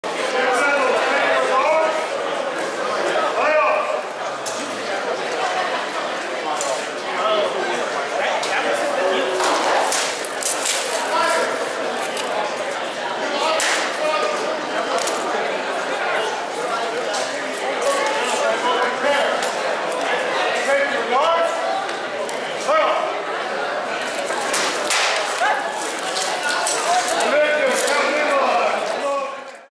Fall Crown 2023, Bout 28: Nikolai v Quenild
Mid-Realm Fall Crown Tourney 2023
Bout #28: Count Nikolai Grigorevich Petrov v Sergeant Quenild of Mercia
Recorded in the Barony of Sternfeld.